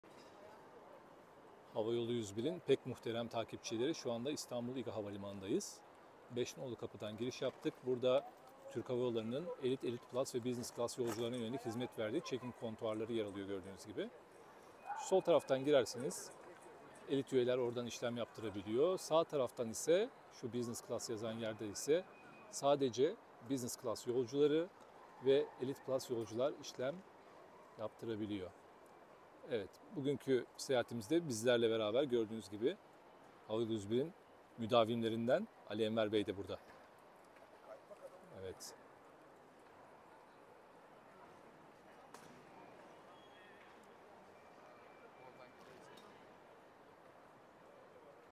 THY Özel Yolcu Check-in Bölgesinin Önündeyiz
Check-in işlemimizi yaptırmak üzere, İstanbul İGA Havalimanı'nda THY'nin çok özel yolcularına ayırdığı bölgedeyiz.